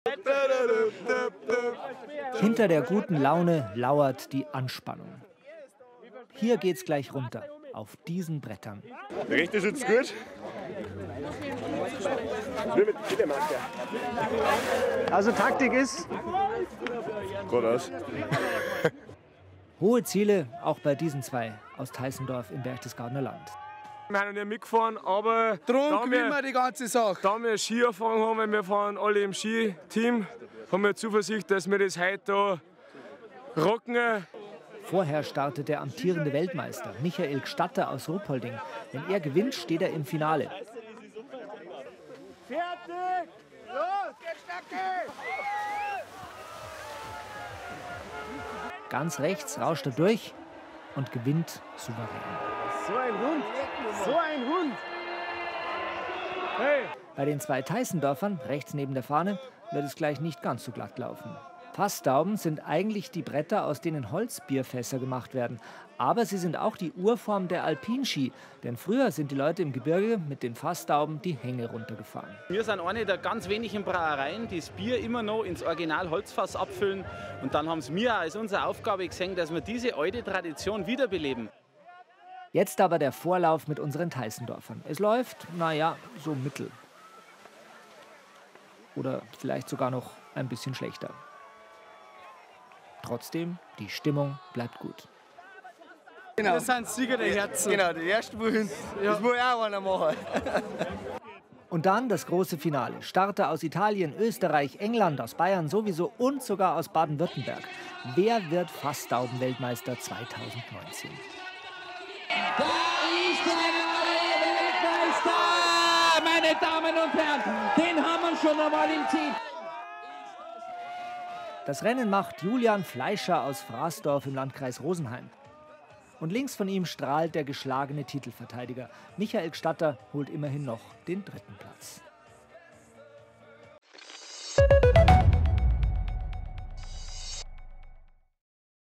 So a Gaudi! Fassdauben-WM in Ruhpolding | BR24
Was früher alle gemacht haben, können heute längst nicht alle - im Gegenteil. Das zeigt die Fassdauben-WM in Ruhpolding am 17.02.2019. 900 Starter und rund 2000 Besucher waren am Westernberg. Die Teilnehmer des Skirennens brauchen vor allem Mut: Sie fahren nicht auf Ski, wie wir sie heute kennen, sondern eben auf Fassdauben.
Fassdauben, das sind eigentlich die Bretter, aus denen die Holzfässer fürs Bier gebaut werden. Aber früher haben sich die Leute in den Bergen diese Bretter an die Füße geschnallt und sind die Hänge runtergesaust - die Urform der Ski war geboren.